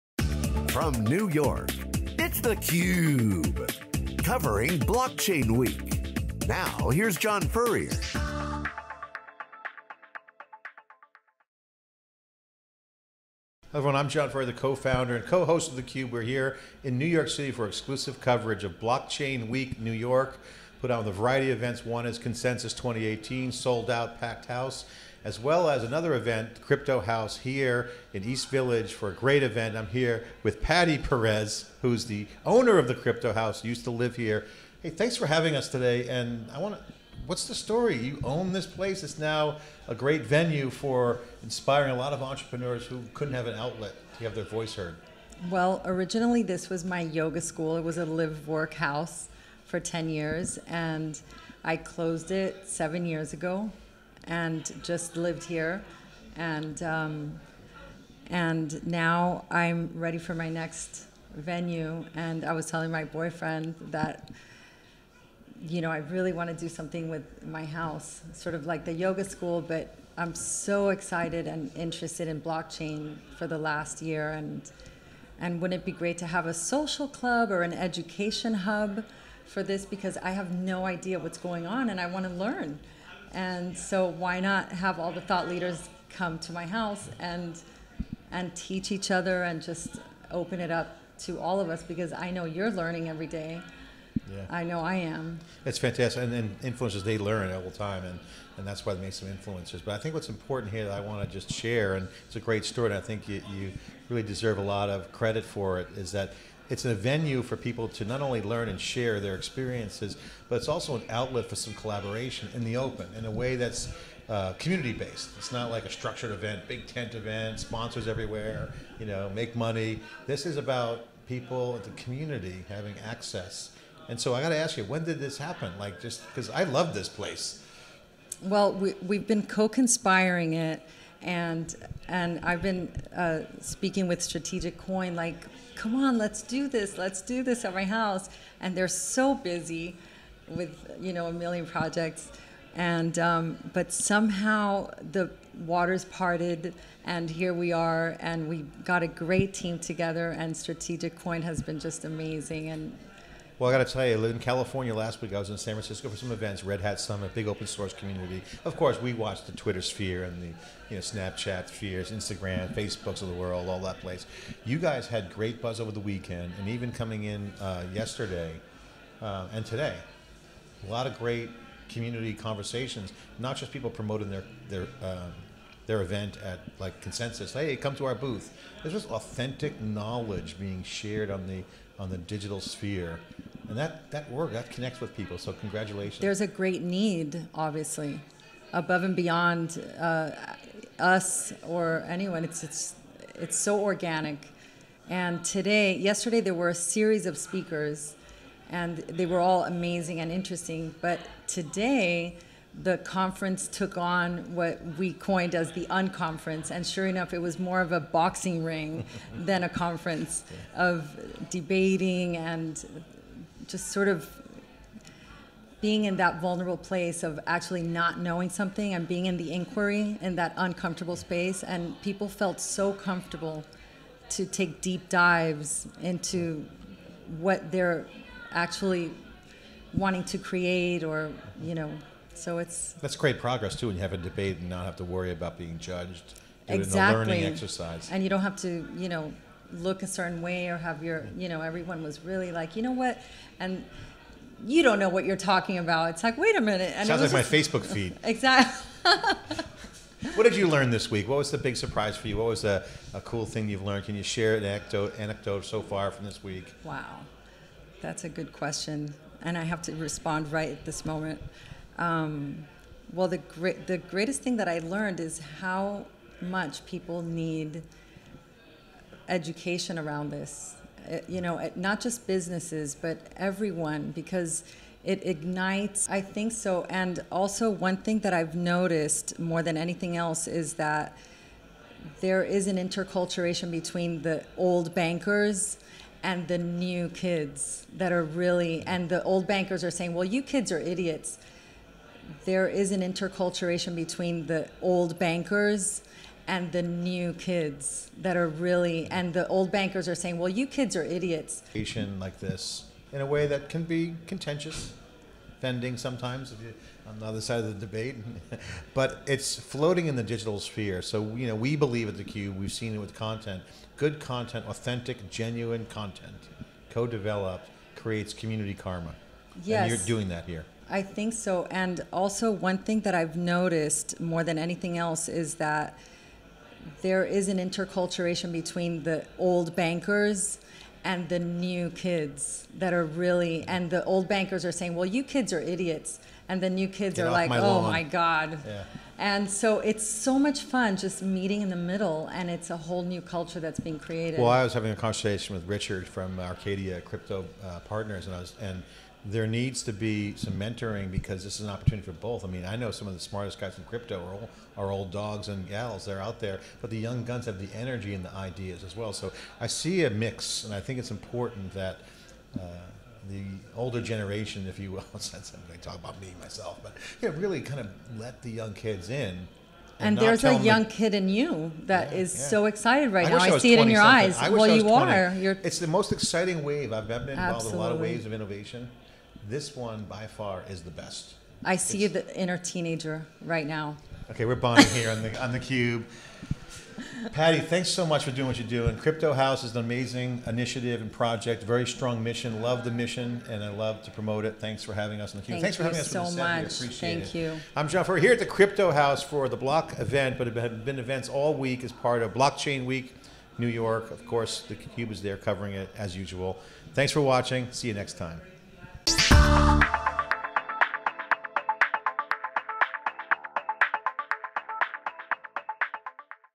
Patty Perez, Cryptohou.se | Blockchain Week NYC 2018
Patty Perez, owner of Cryptohou.se joins SiliconANGLE Co-Founder John Furrier (@furrier) as part of our coverage of Blockchain Week NYC 2018